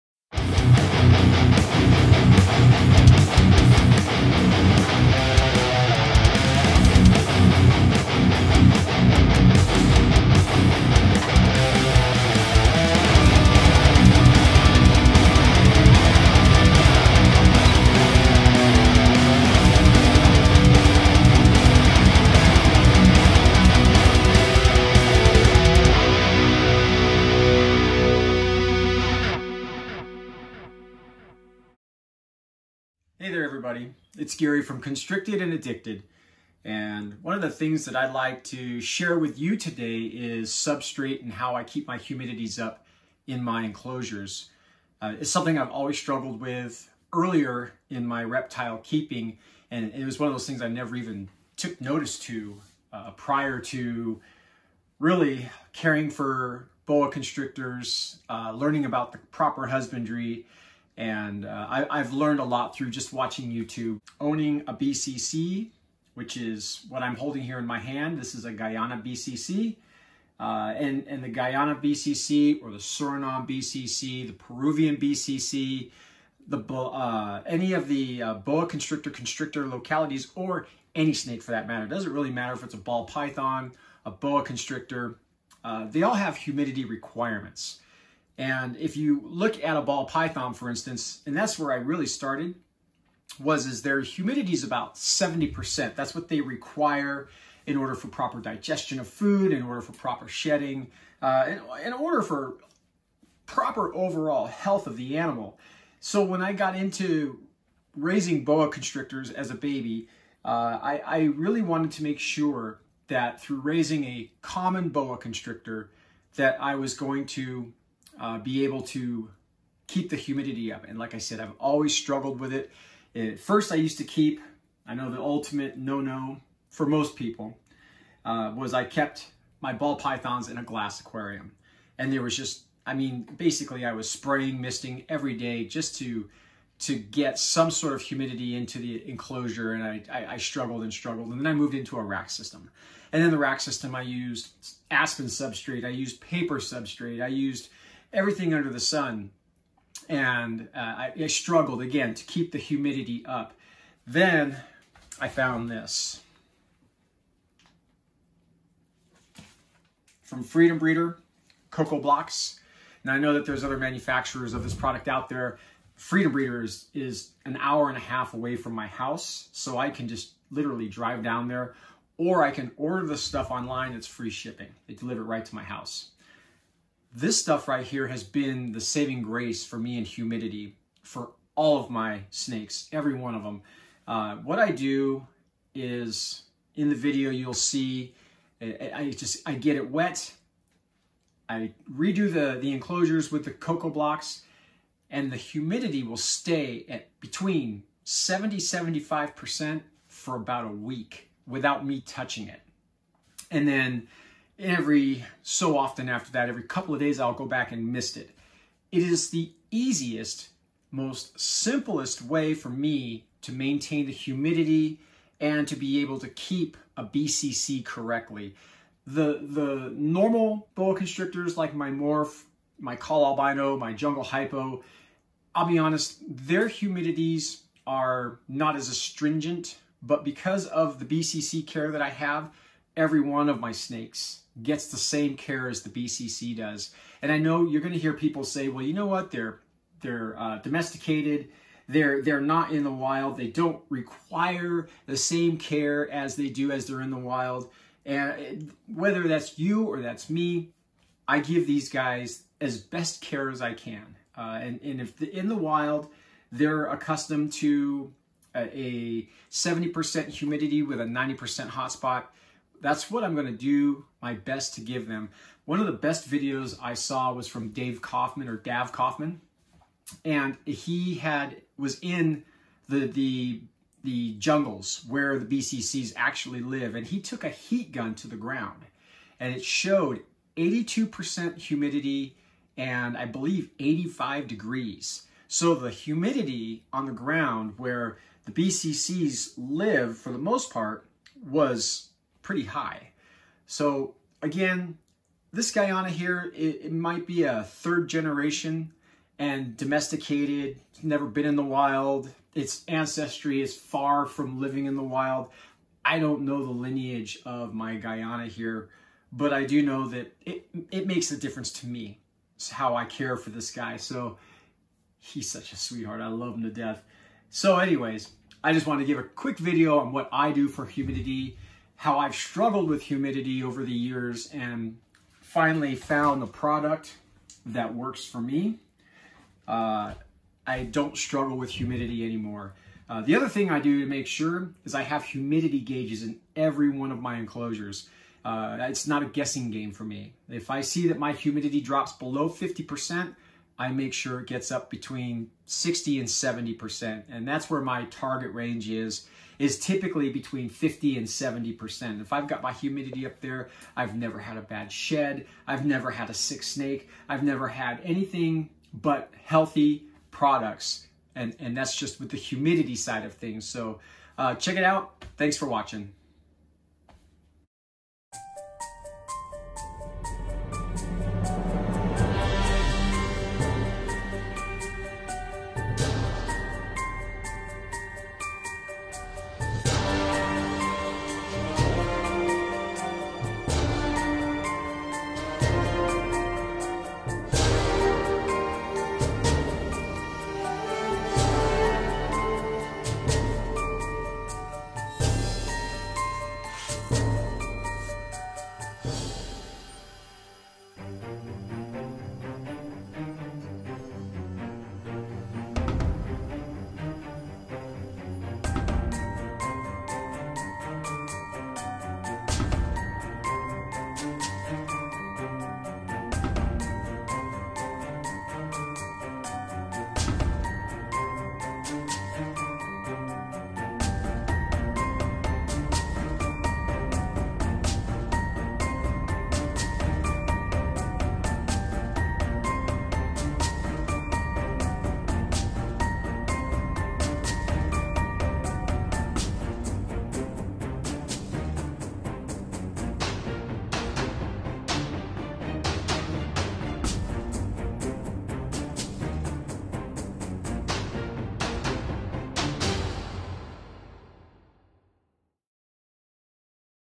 My Humidity Solution
I have struggled with maintaining humidity and this is the best substrate I have found to keep it.